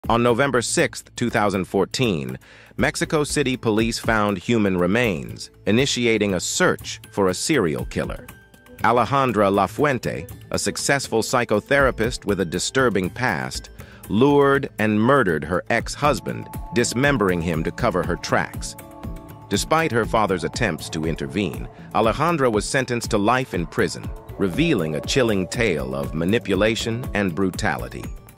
A monster in the body of a beauty! The Chilling Case of Alejandra Lafuente | True Crime Documentary
The story delves into a chilling narrative of Alejandra Lafuente, a deceptive psychotherapist who turns out to be a serial killer. Her meticulously planned murders, including that of her ex-husband, reveal a dark and manipulative side. Despite attempts to cover her tracks, Alejandra's true nature is unveiled, leading to a life sentence for her crimes, portraying the chilling consequences of unchecked evil.